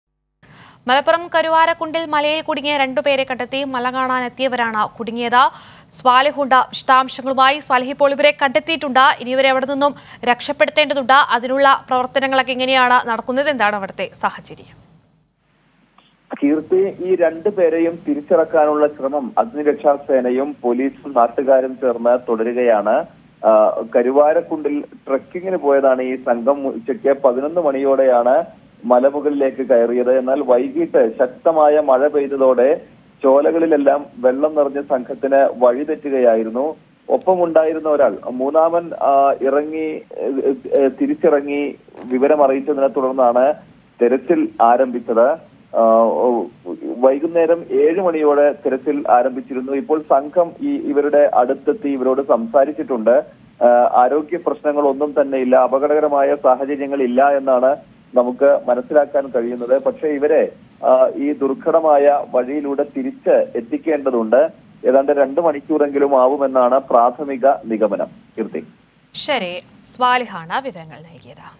പാലക്കാട് കരുവാരക്കുണ്ട് മലയിൽ കുടുങ്ങിയവരെ കണ്ടെത്തി | MALAPPURAM
മല കാണാനെത്തിയവരാണ് തിരിച്ചിറങ്ങാൻ കഴിയാതെ കുടുങ്ങിയത്.ഫയർ ഫോഴ്‌സും പോലീസും സ്ഥലത്തെത്തിയാണ് കണ്ടെത്തിയത്